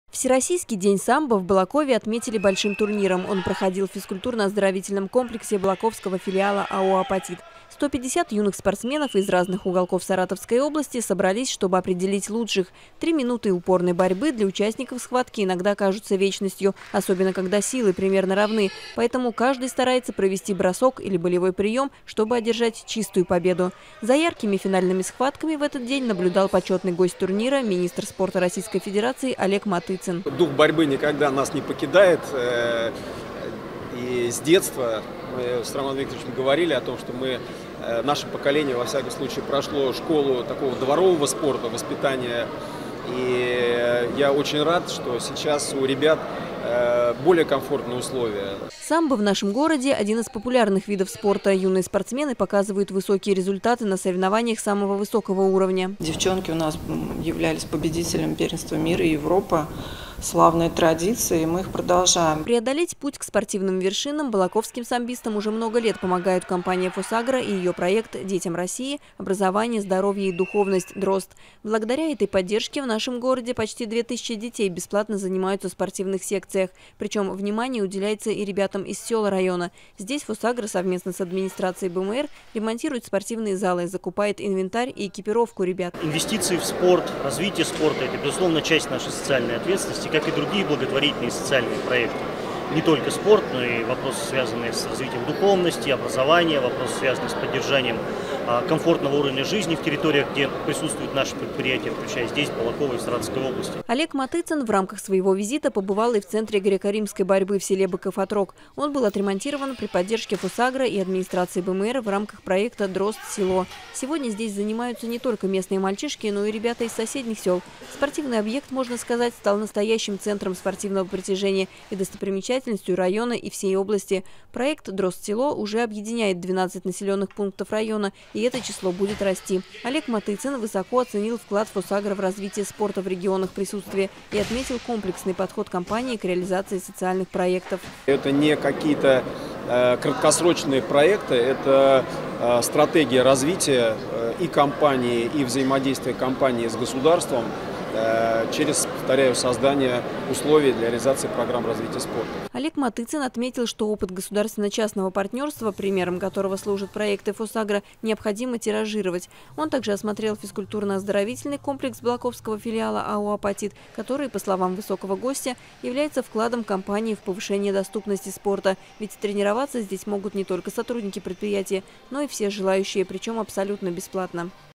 Министр спорта РФ Олег Матыцин побывал на турнире по самбо в Балакове
Всероссийский день самбо в Балакове отметили большим турниром. Он проходил в физкультурно-оздоровительном комплексе БФ АО «Апатит». 150 юных спортсменов из разных уголков Саратовской области собрались, чтобы определить лучших. За яркими финальными схватками в этот день наблюдал почётный гость турнира - министр спорта РФ Олег Матыцин.
Как высокий гость оценил спортивные достижения юных борцов и вклад компании "ФосАгро" в социальное развитие Саратовской области? Об этом - в нашем сюжете